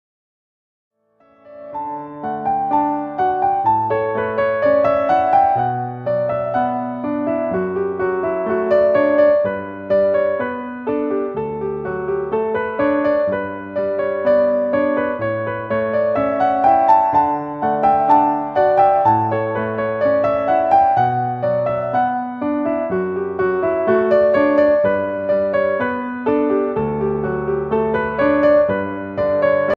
knob assemble
Elevate Your Cookware Brand with Our Pot Lid Knob💓

Easy assemble to ensure customers can do it well

Sleek silicone edge that stands up to high temperatures with ease

Perfect blend of style and functionality

If you are interested, pls contact Susan's whats'app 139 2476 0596.